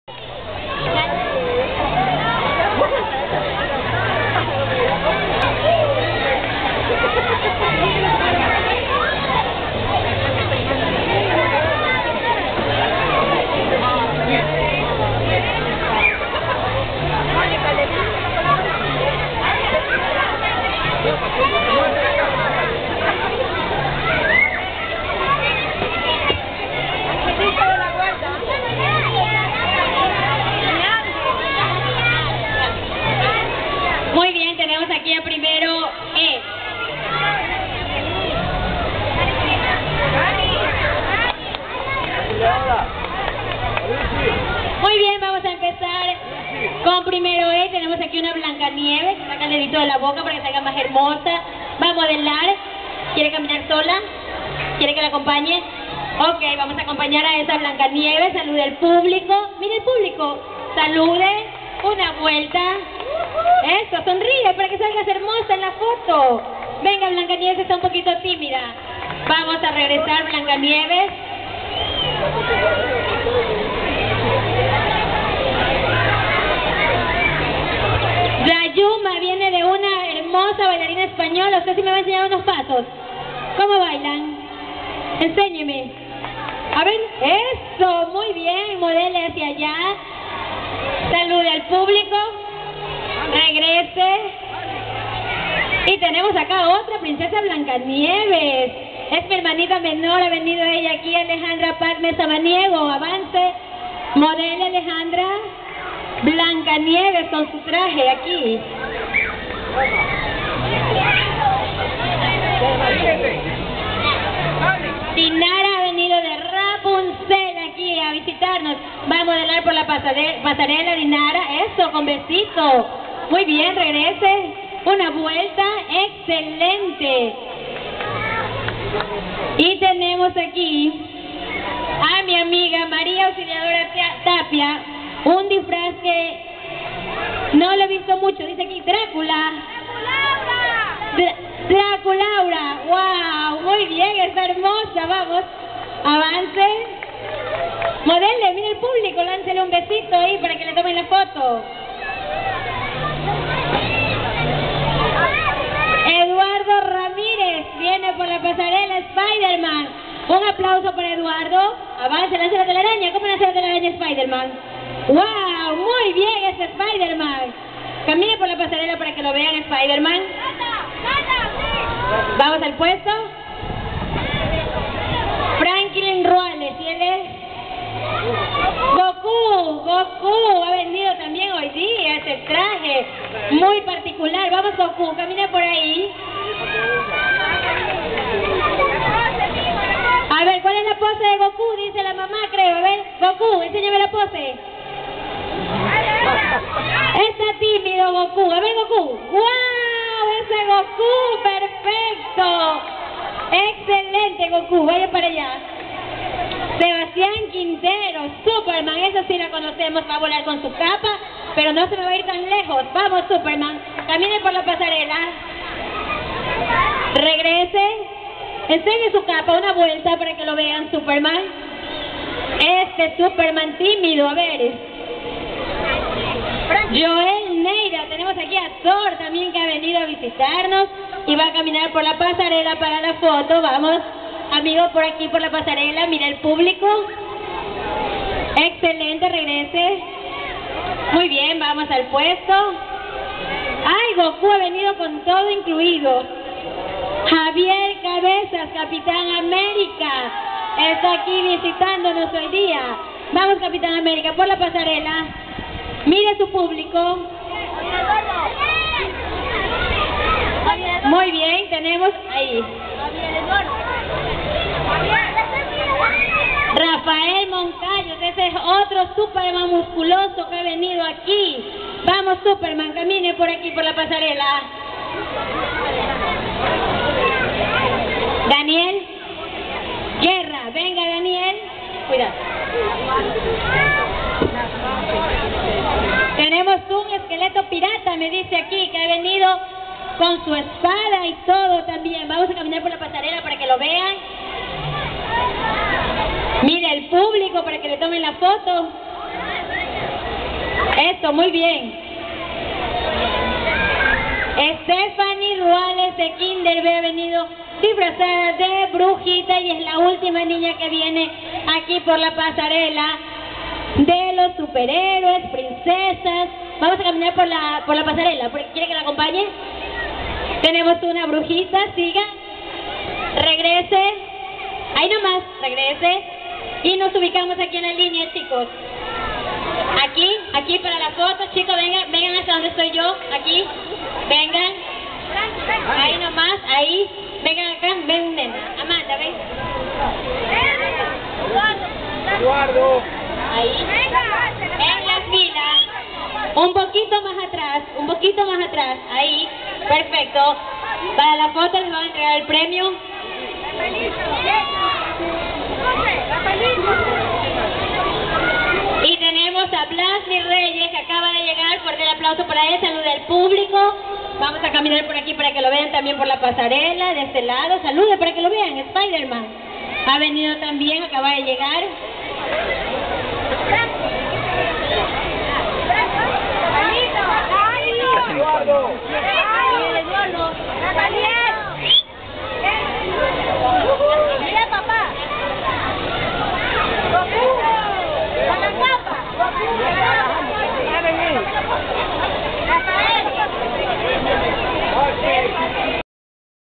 Kermesse Costume Parade
Mi hija Padme en su desfile de Disfraces en su Colegio Americano.